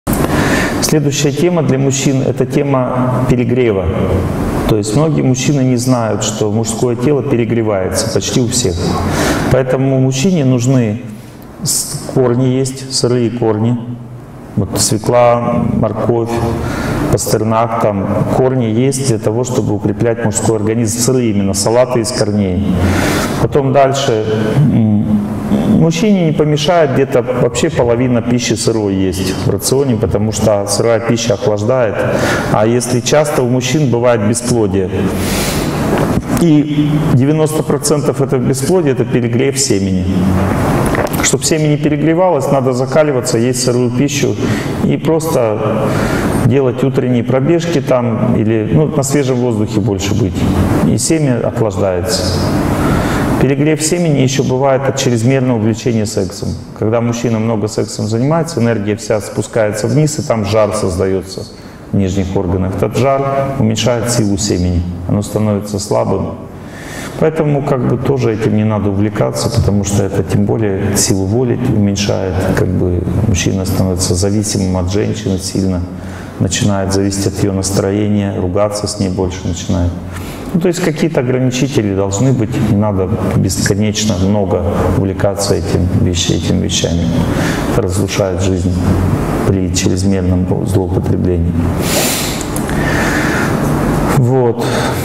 Торсунов О.Г. О перегреве
В этом видео Торсунов Олег Геннадьевич говорит о перегреве
Полную версию видео можно посмотреть по ссылке: